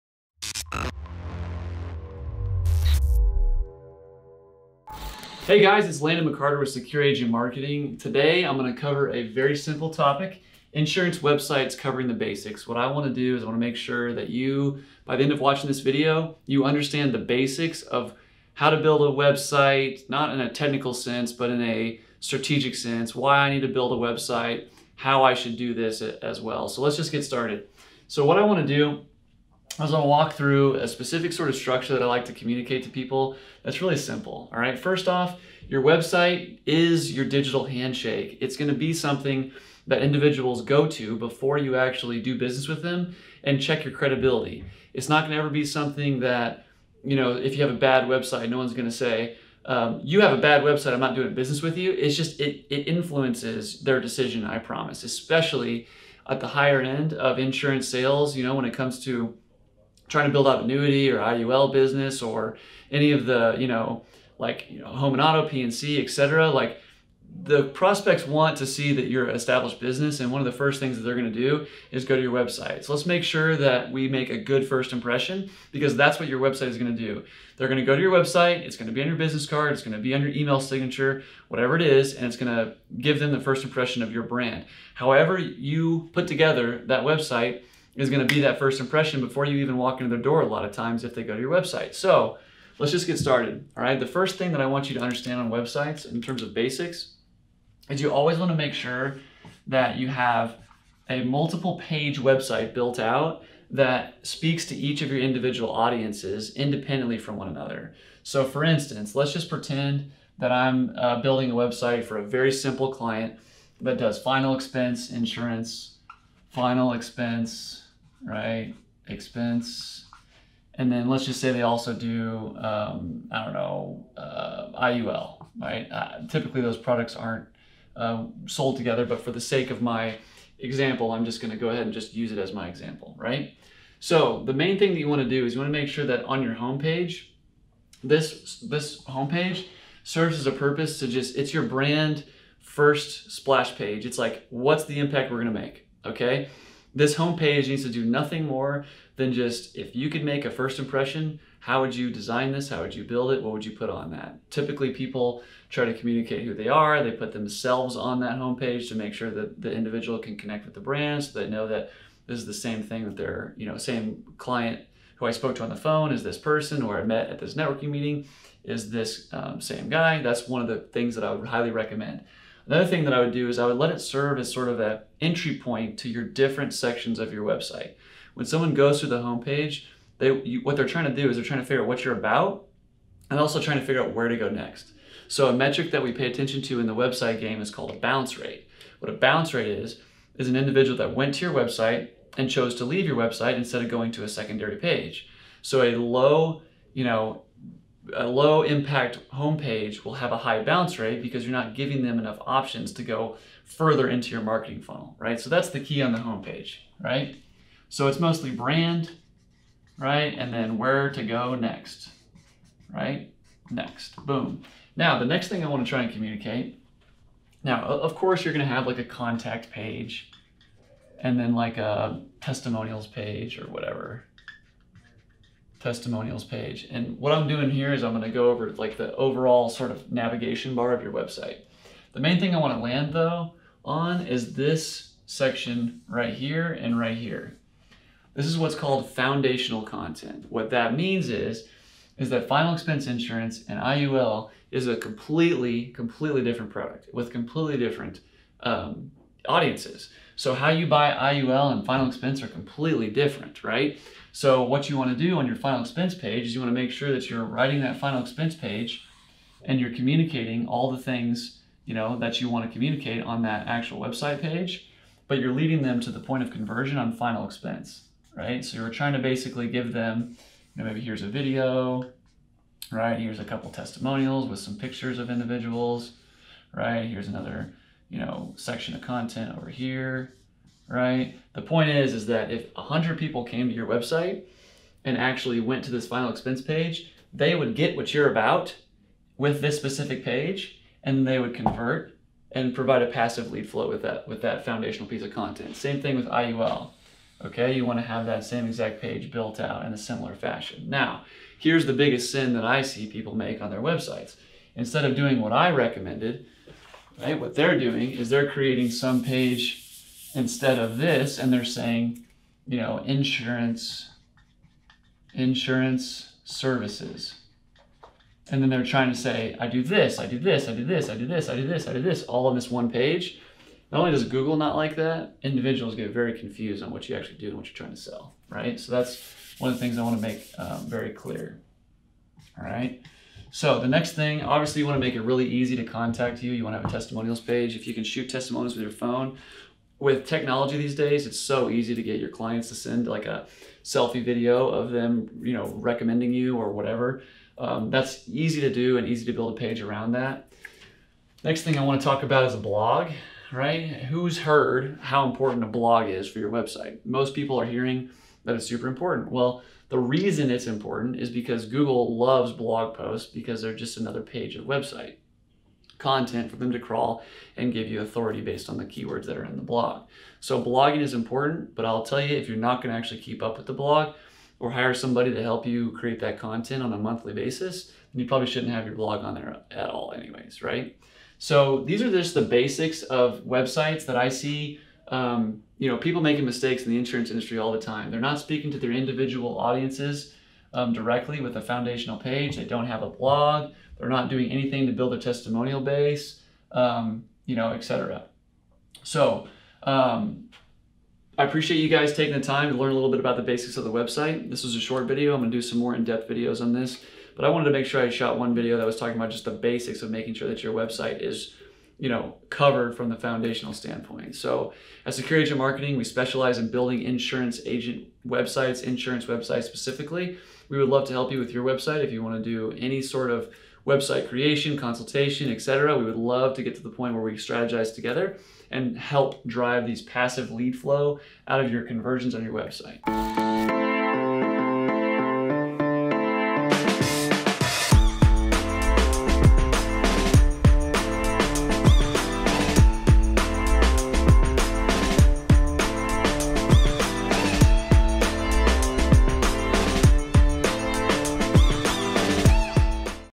Insurance Websites: Covering The Basics
A website’s purpose can vary from one insurance company to the other, but one thing always remains the same.

Prospects, potential employees, and anyone looking to check you out will be visiting your website prior to working with you.

You must make sure your website carries the brand and professionalism that you do.

Your website serves as a digital resume. It must be excellent, be found, and communicate who you are as a brand.